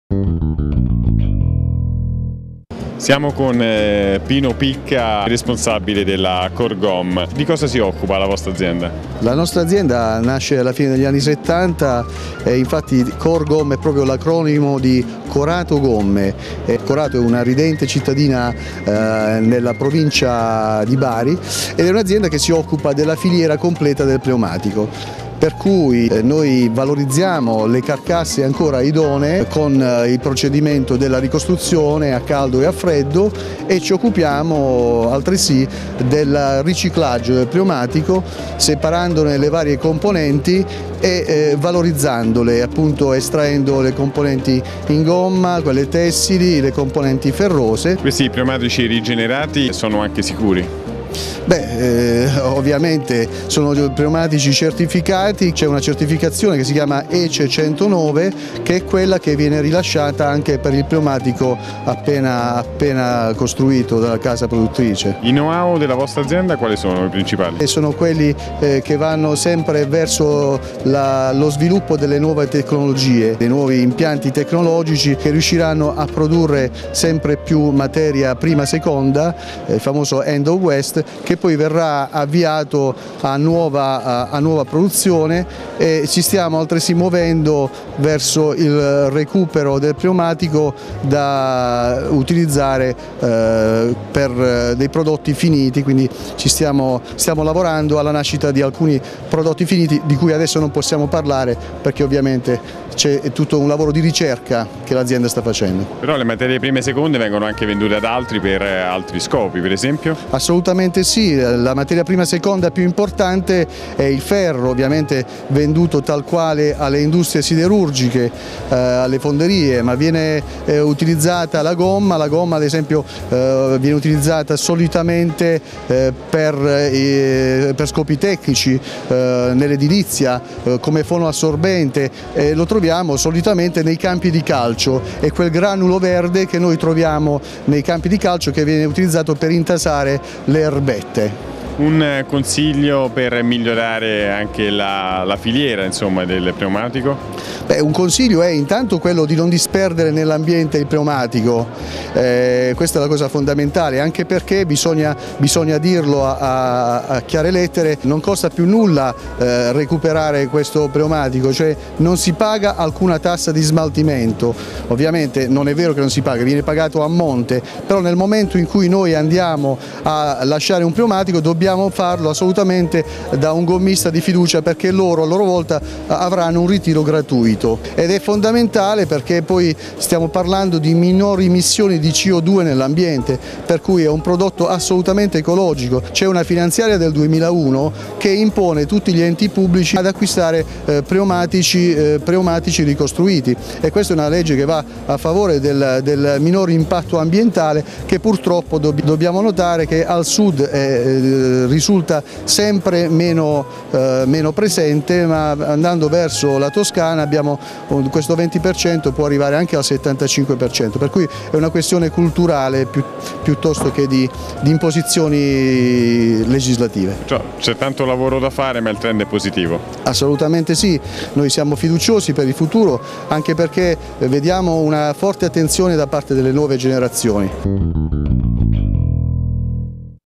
ECOMONDO2015: CORGOM
Teleambiente intervista Pino Picca, responsabile CORGOM.